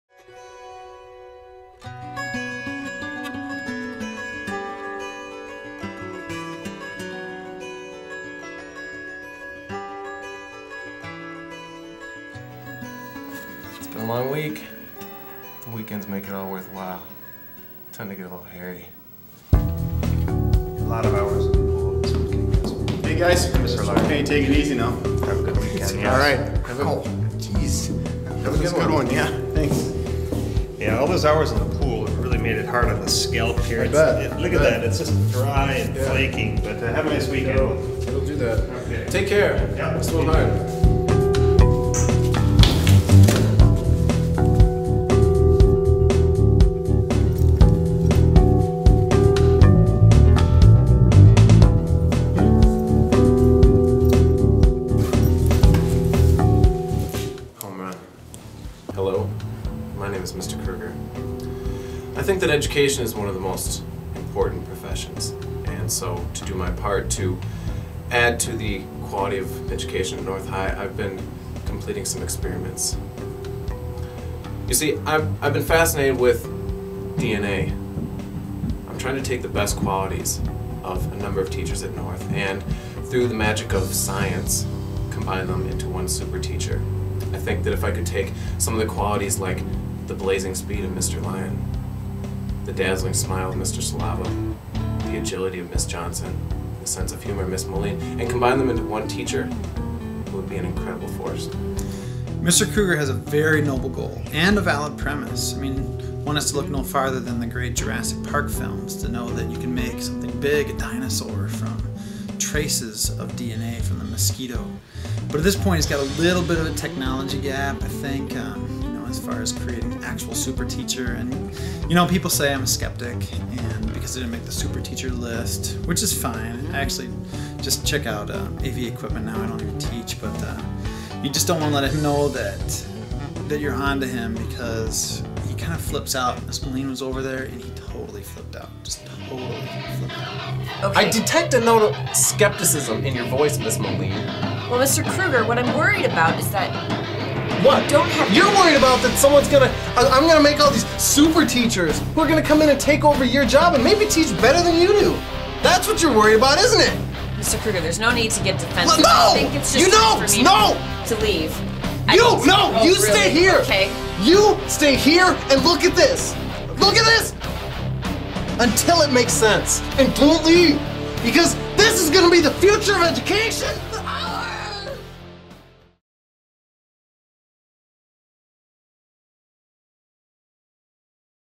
The Secret Lives of Teachers - SuperTeacher Maker
Mr. Krueger shares his passion for teaching and science by creating the ultimate "Super Teacher"